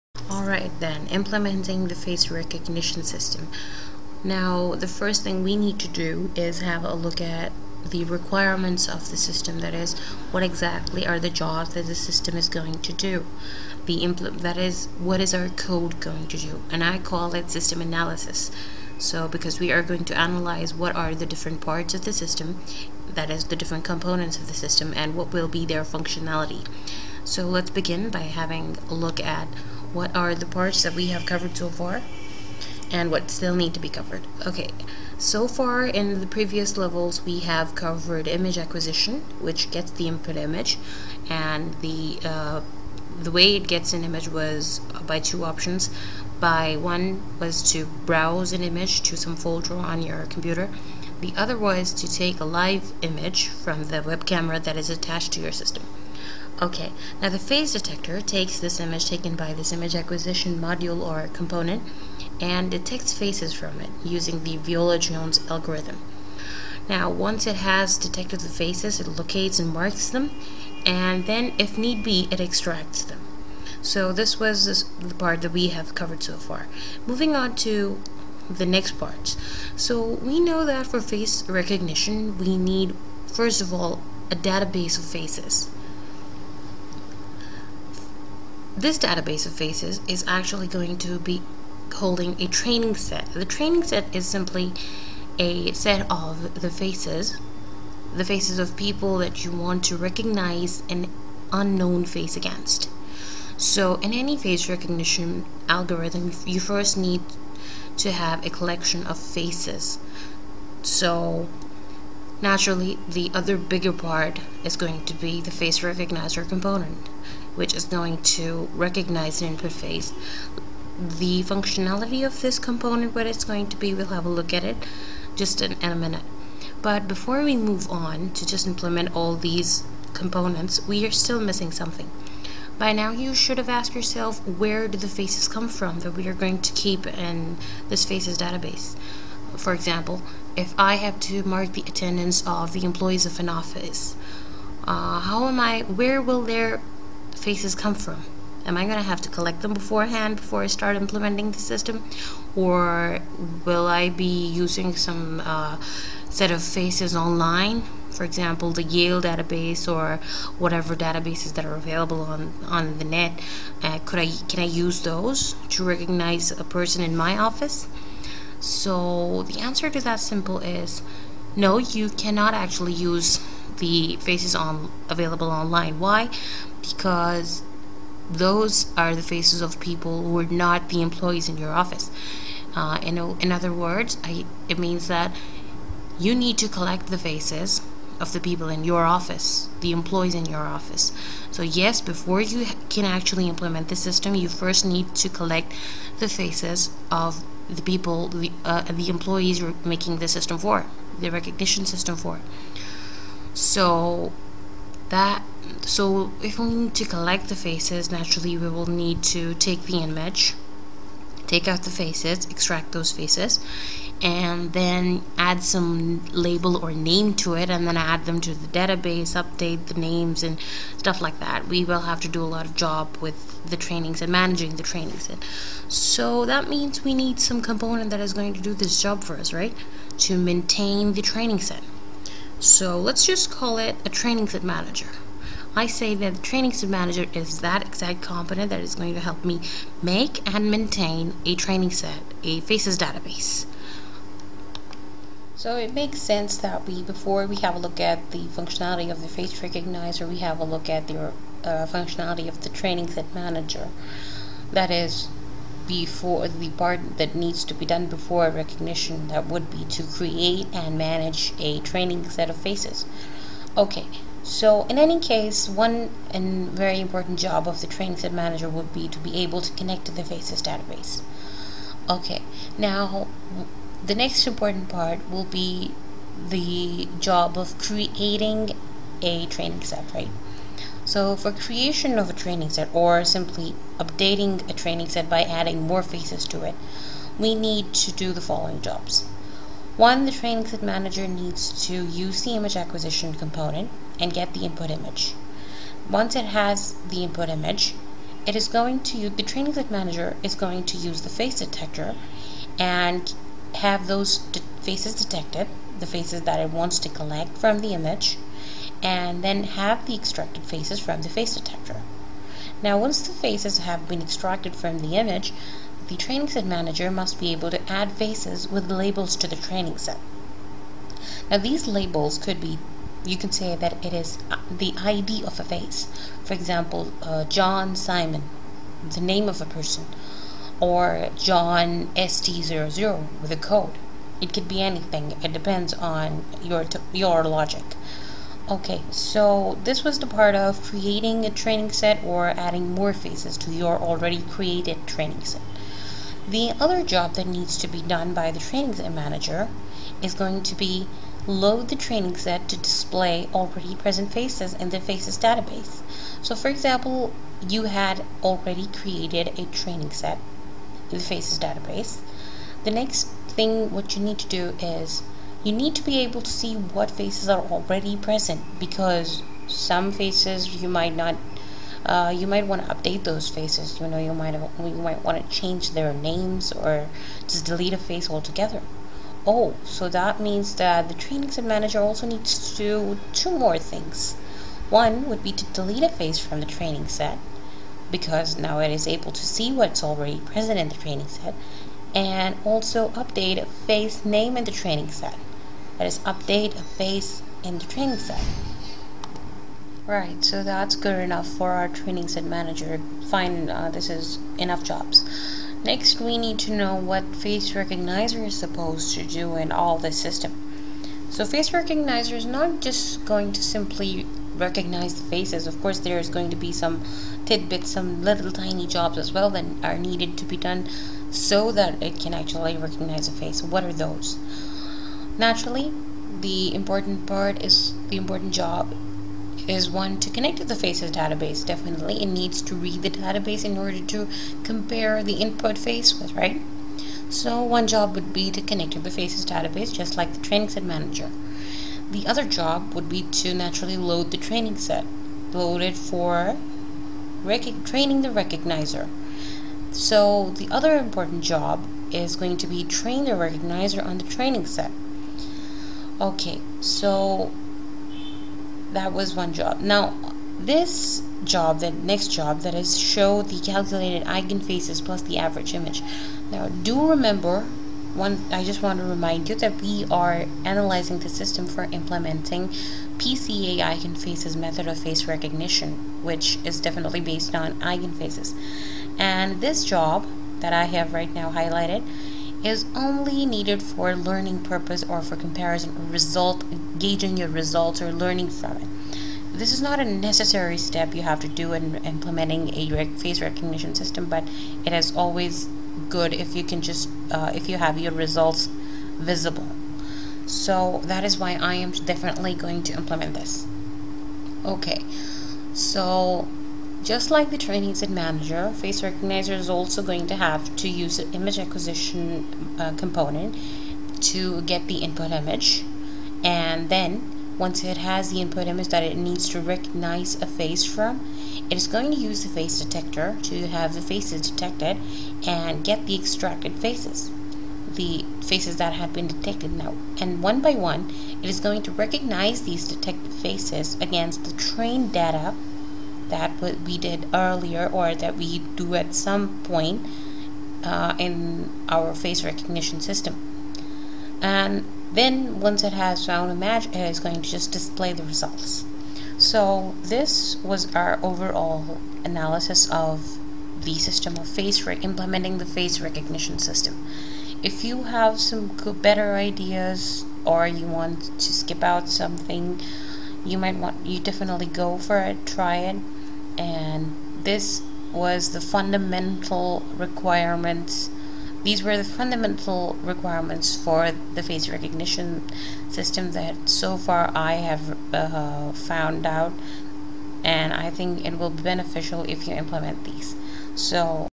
Analyze & Design a Face Recognition System (3_5)
Face Recognition Tutorial Series:
Level 4c -- PART I -- Analysis & Design of Face Recognition System
Video Description:
-Decide the functionality that the face recognition system must do.
-Group functions to make components and
-Decide how these components interact with each other to build and do work of face recognition system.